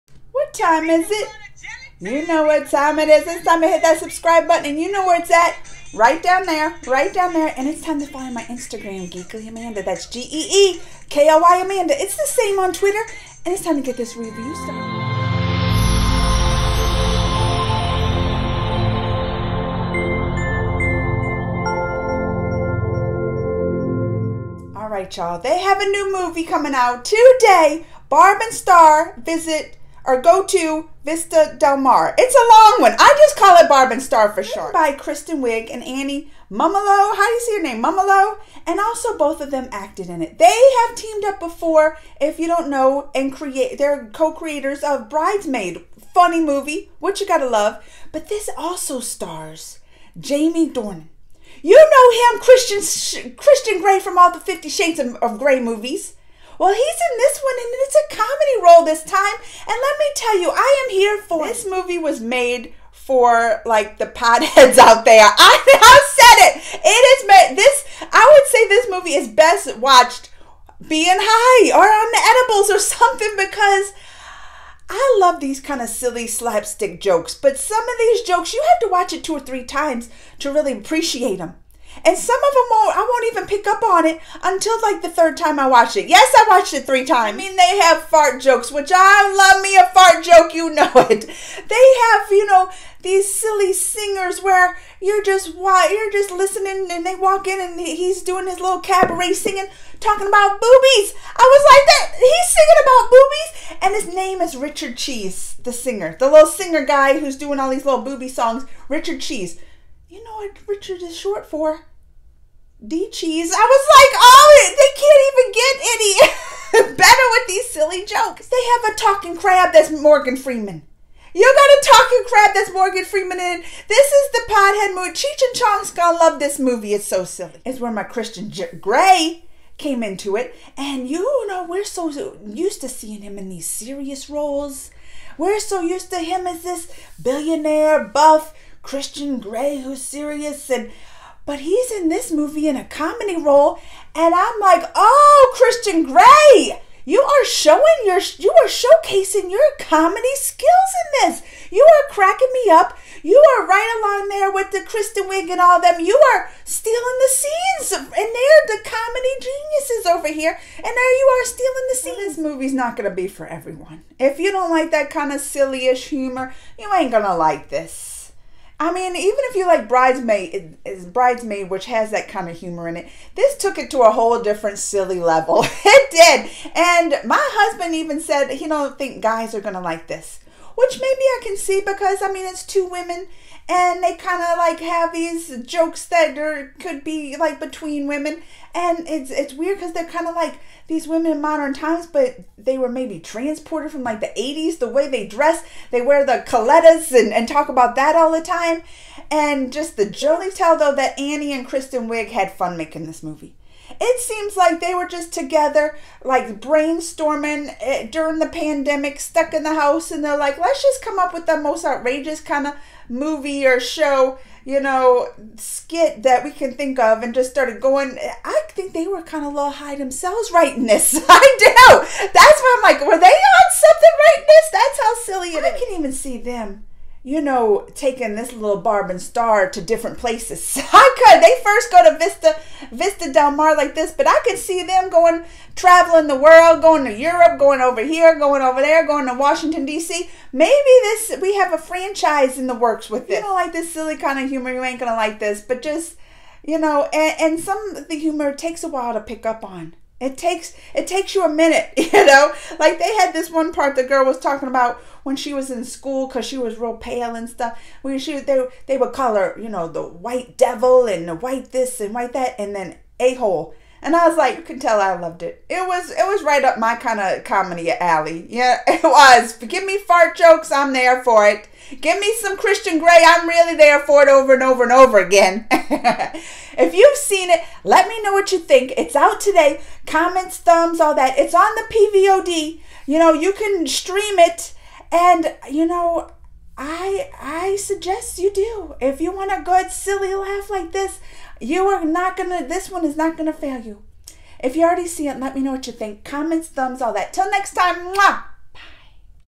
Barb And Star Go To Vista Del Mar MOVIE REVIEW
Barb And Star Go To Vista Del Mar is now available to stream, written and directed by Kristen Wiig and Annie Mumolo and it even has Jamie Dornan, who we know from the 50 Shades movies!!!